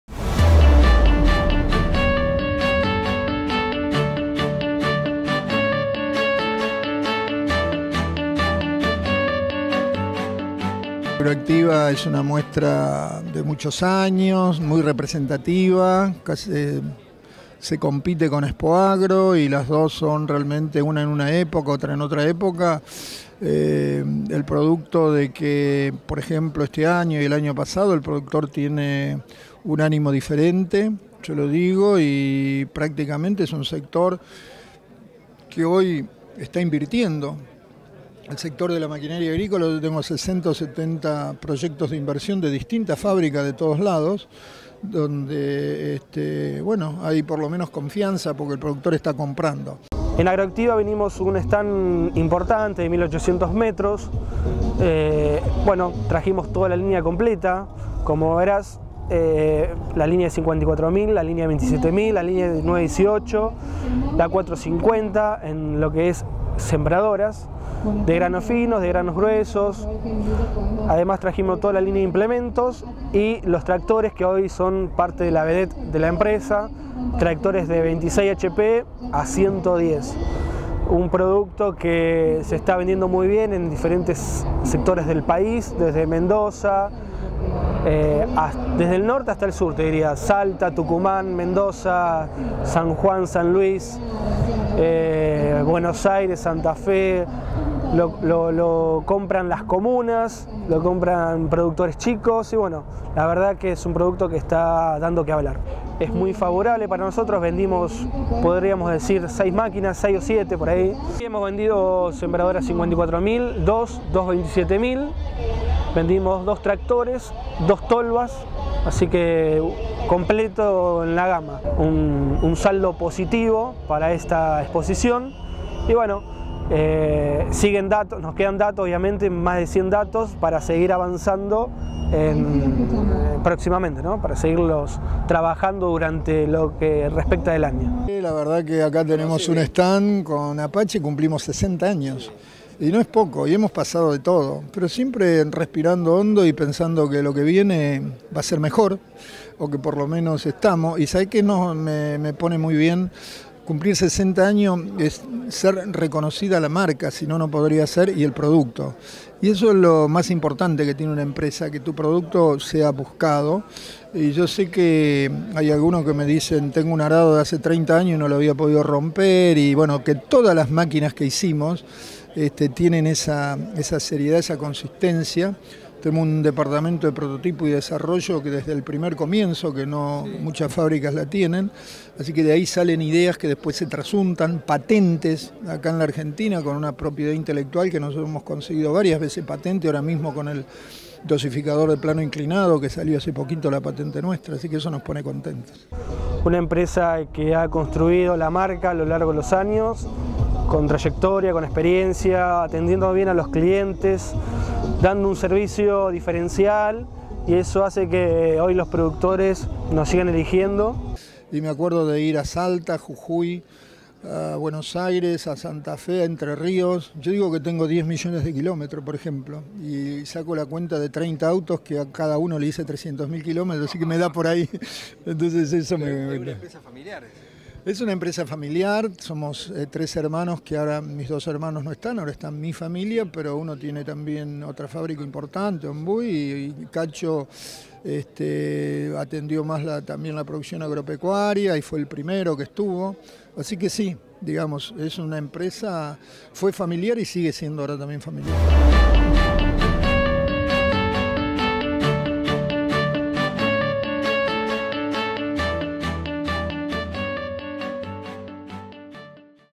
Apache presente en Agroactiva 2017
Carlos Castellani, Presidente APACHE SA
Lucas Alvarez, Gte. de Márketing, Apache SA